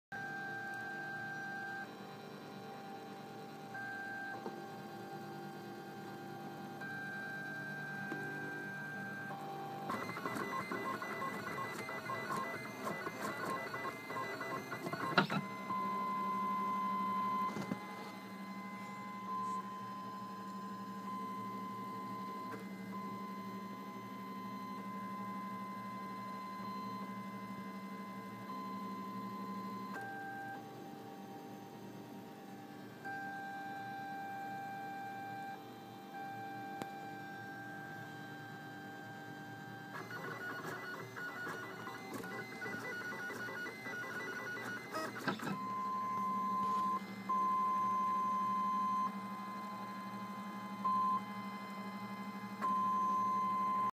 Red's Self-Sourced Ultimaker - first print
After some clogging, blobbing, and whatnot, here's the first real print. The Z steps are wrong, thanks to my metric leadscrew, but after some fiddling in the firmware, it should work as expected.